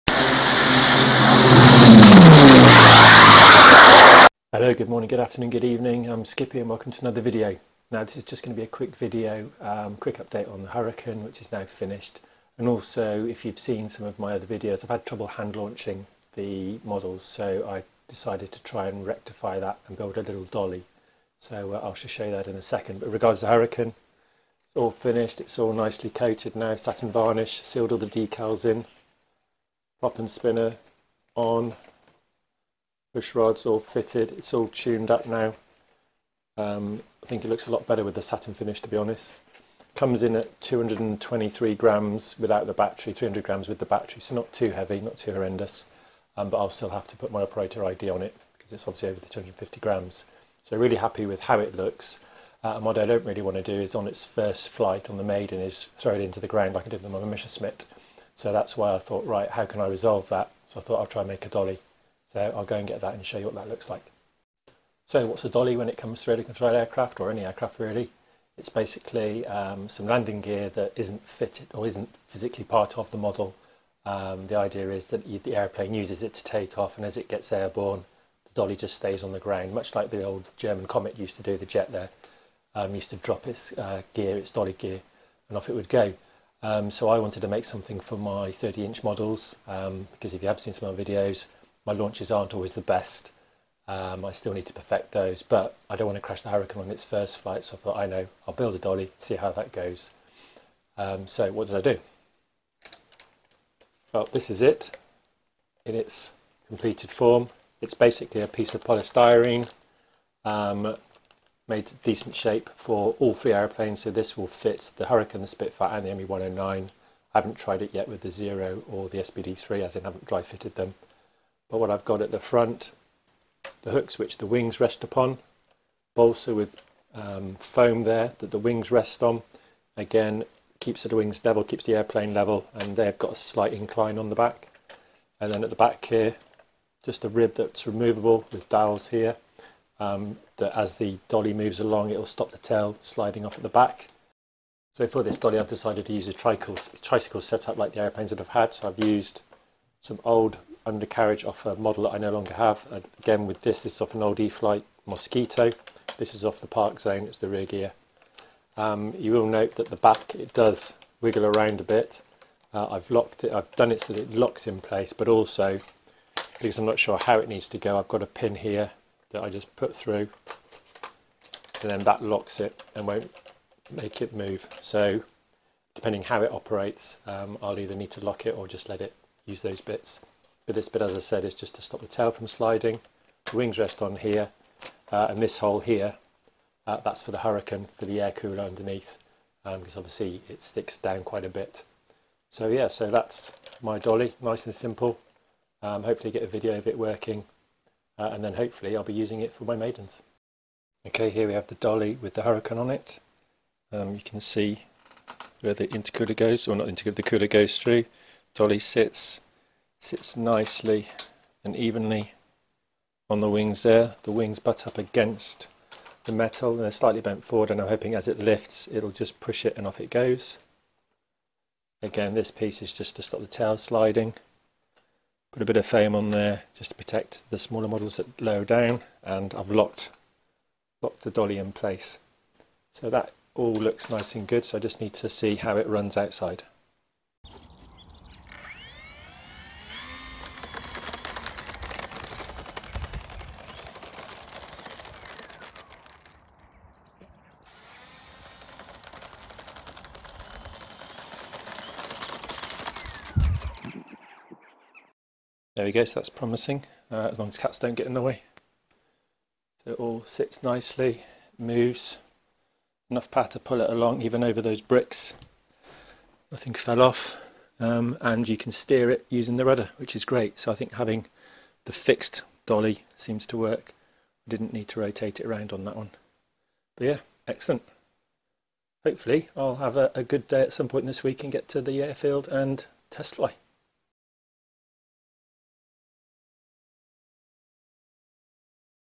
SKippy's Dumas Hawker Hurricane RC Conversion update and RC Dolly build - Part 4
A quick video update now that the Hurricane is now ready to fly having received the satin varnish coating to seal the decals and offer more protection from water when landing on wet grass and all the little bits added (no aerial). Also, in an attempt to prevent the same crash on first flight like I had with the Me109, I've made a dolly that will hopefully enable a more controlled launch into the air in a more conventional manner. As none of my 30" models have landing gear this will hopefully be useful for all 5 of the current collection and I have already tried taxying with the Hurricane, Spitfire and Me109 outside my house. The true test will be on maiden day when I give it more power!! The tarmac surface isn;t great at the flying site so it may still be a challenge.